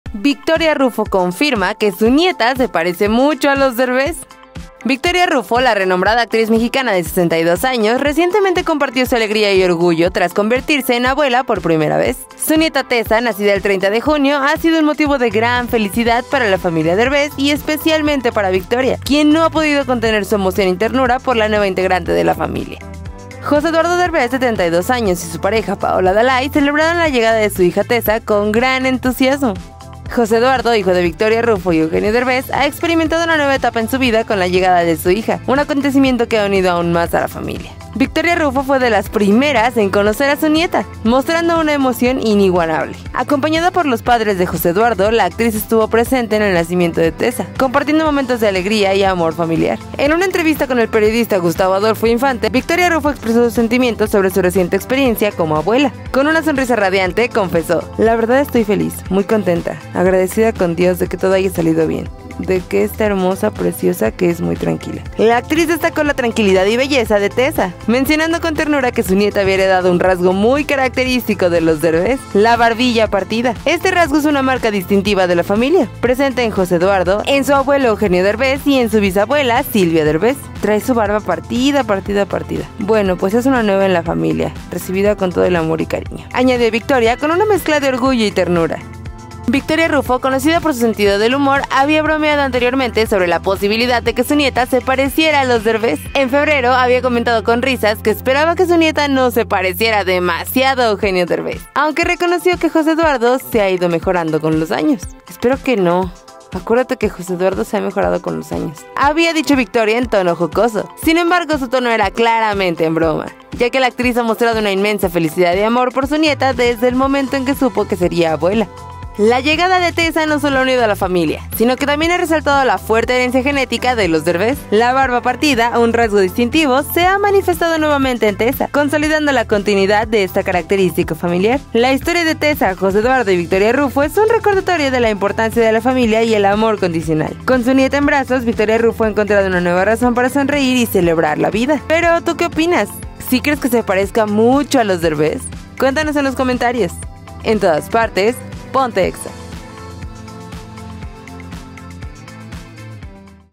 Victoria Ruffo CONFIRMA que su nieta se parece MUCHO a los Derbez
VictoriaRuffo reveló que su ieta tiene un rasgo muuy Derbez 😱🤭 ¿A quién se parecerá?